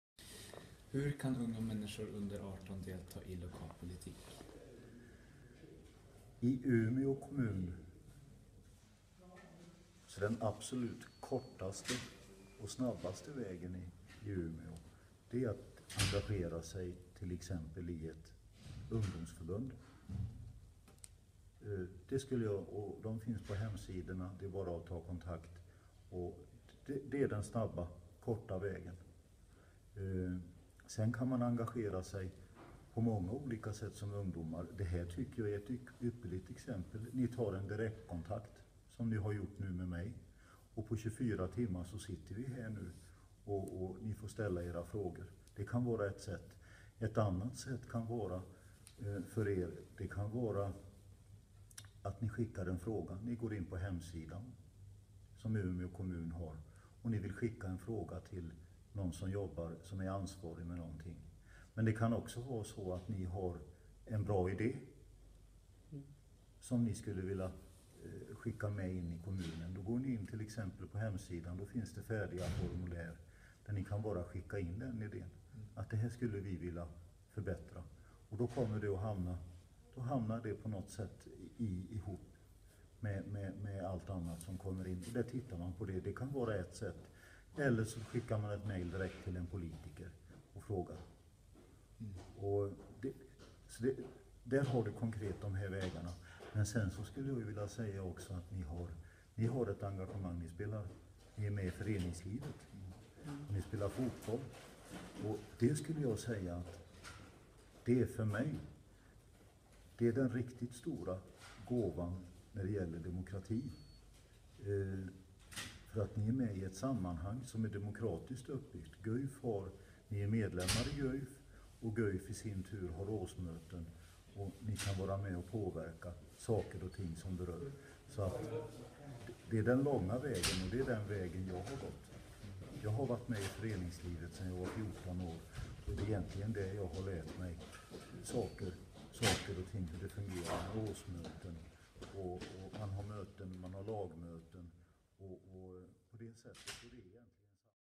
Intervju Flensburg
Interview with local politician about youth influence in the EU, as part of the Erasmus+ project See & Act, Active Citizens Together.